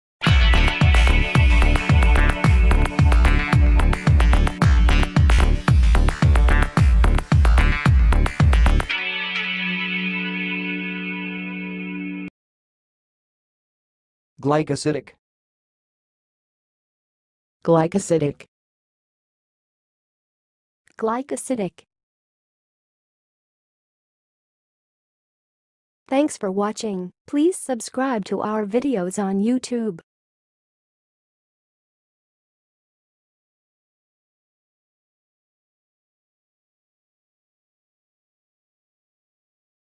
How to say "glycosidic"! (High Quality Voices)
Watch in this video how to say and pronounce "glycosidic"!